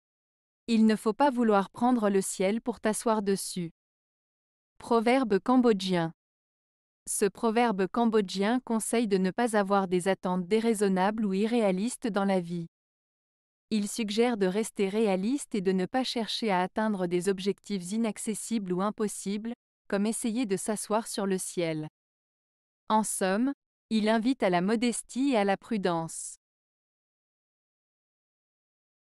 Il ne faut pas vouloir prendre le ciel pour t'asseoir dessus. - Proverbe Cambodgien
Ce proverbe cambodgien conseille de ne pas avoir des attentes déraisonnables ou irréalistes dans la vie. Il suggère de rester réaliste et de ne pas chercher à atteindre des objectifs inaccessibles ou impossibles, comme essayer de s'asseoir sur le ciel. En somme, il invite à la modestie et à la prudence.